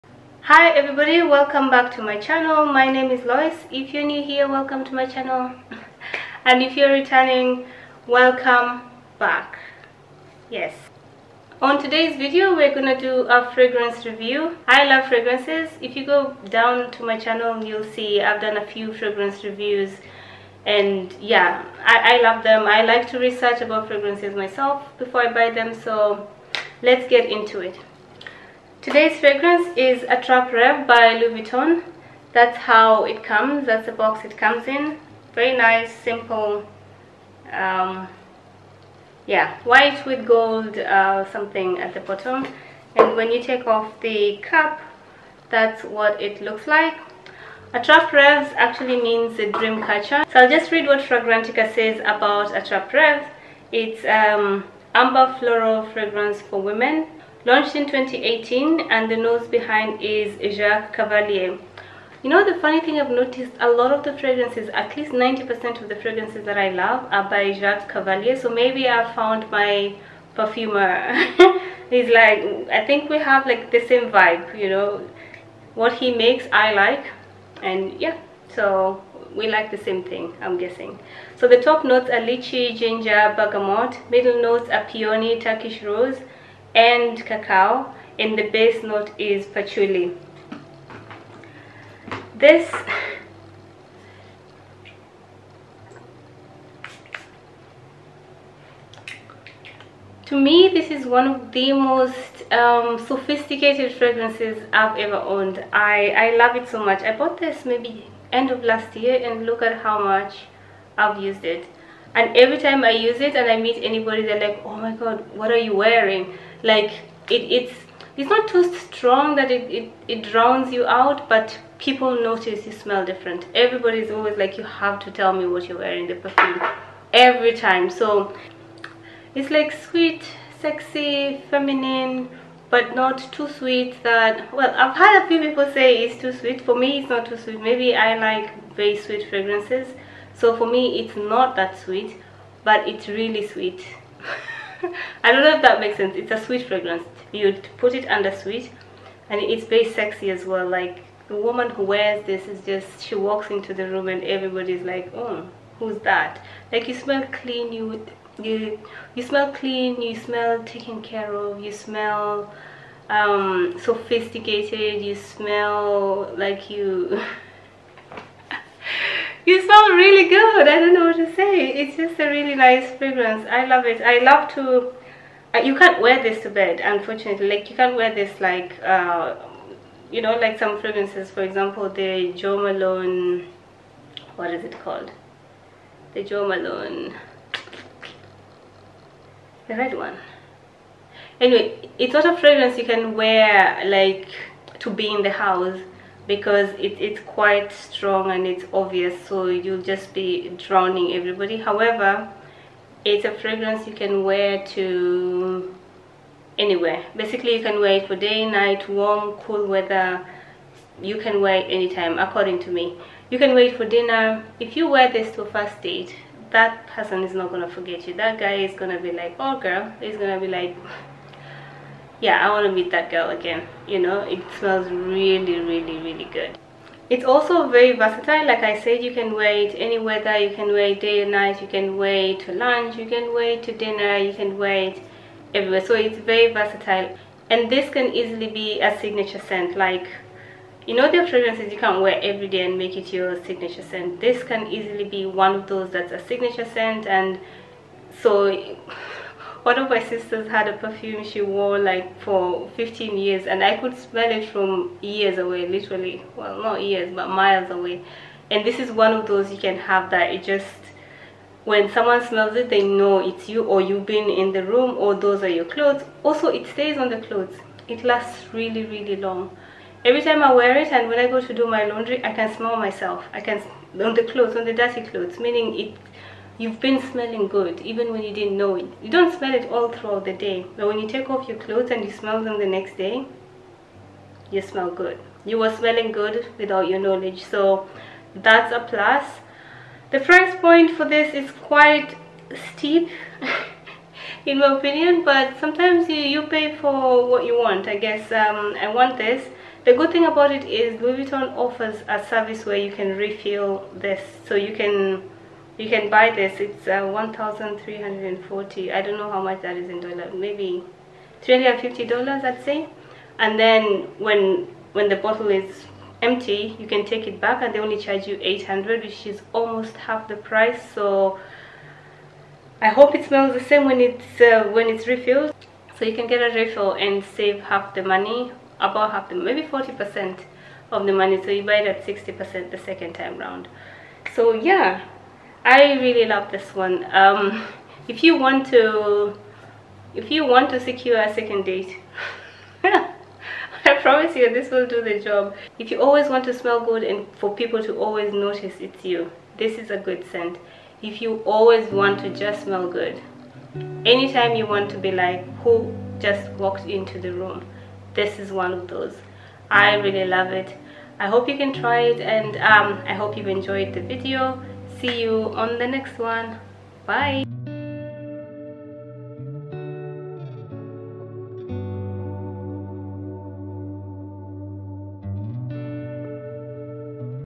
Louis Vuitton's Attrape Reves Review | Loice Lamba
This video is a fragrance review of Attrape Reves by Louis Vuitton.